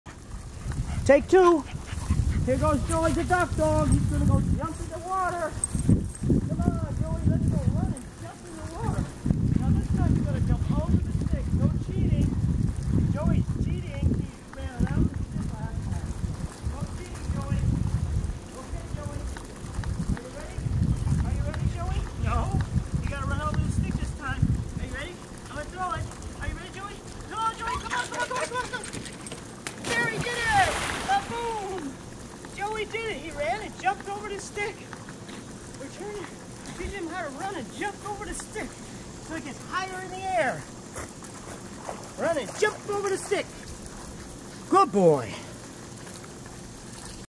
Joseph the Corgi Dog
Little Joe went to camp the other day, and had a furn time jump'n in the  swimm'n hole.  We missed the big show in San Antonio, but we'll make it later this  year.  Joey competes in Rally and Obedience next week end.  I'm teaching  Joseph to get some air at the end of the dock to give him another foot or two.  Go  Joey Go !!!  Have  Fun !!